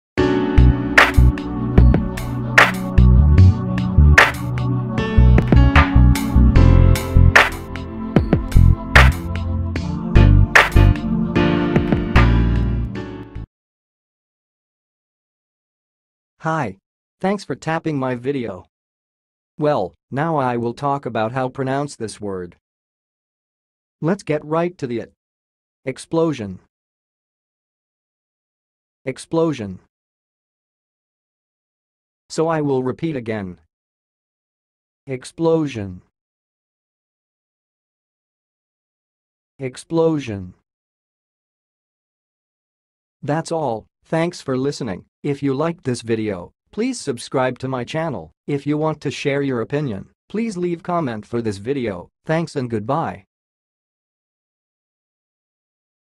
EXPLOSION - HOW TO PRONOUNCE IT!?
Watch how to say and pronounce "explosion"!